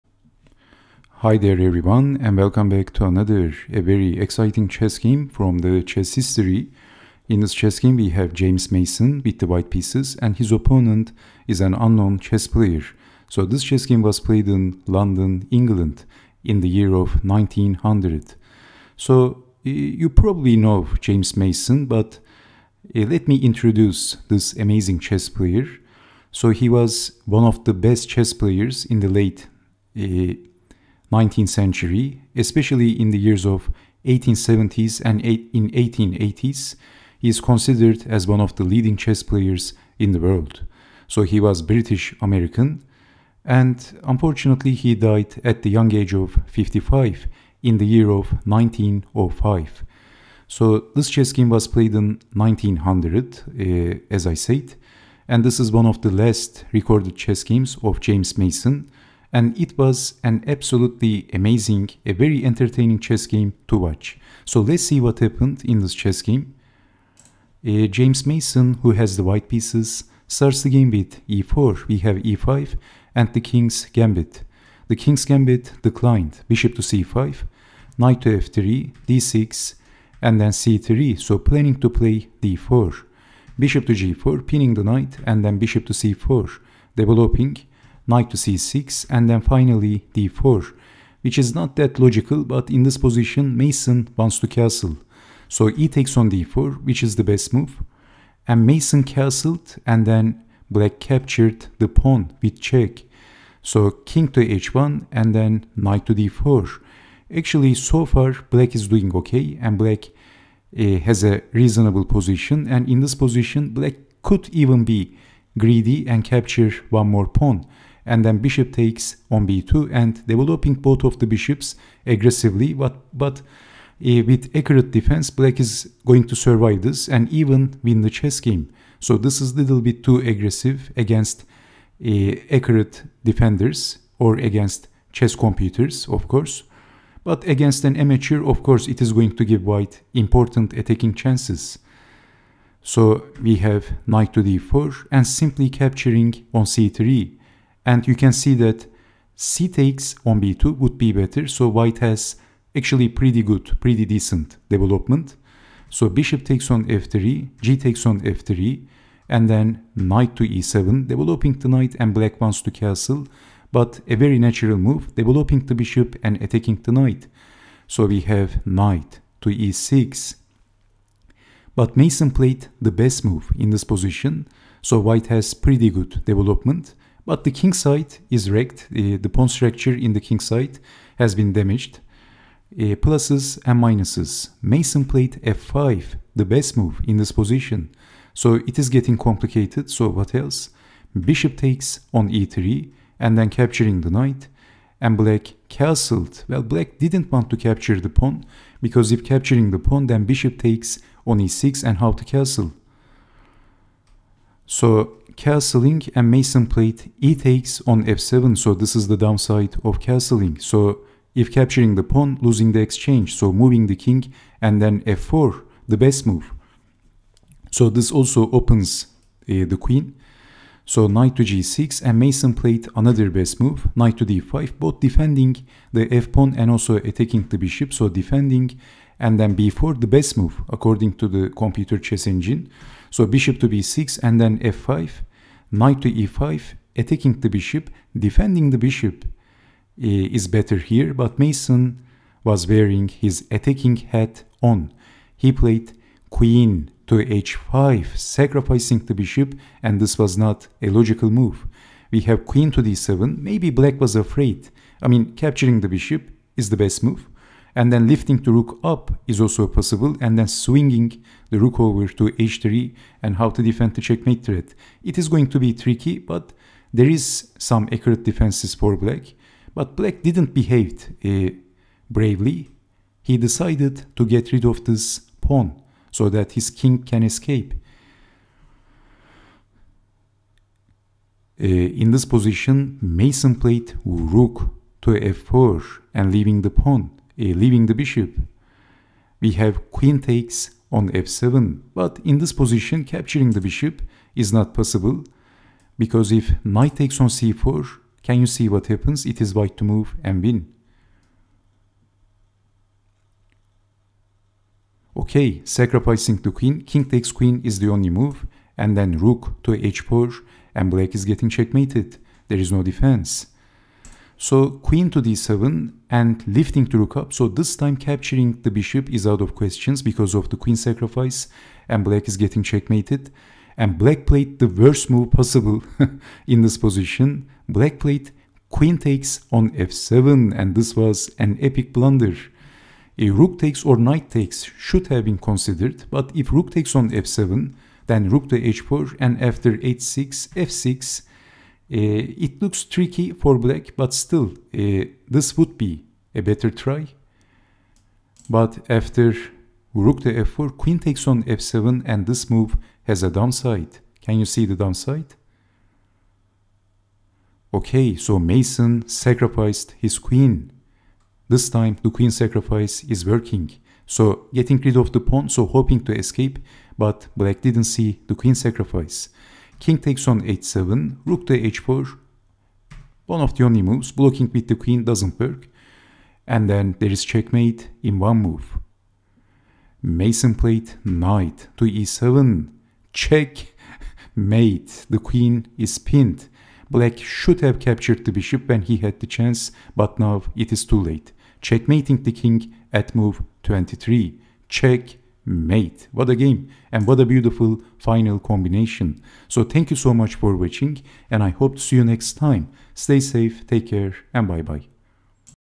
What a Game: James Mason vs NN - London (1900)
James Mason (White)

NN (Black)

London England (1900)

King's Gambit Declined. Classical Variation General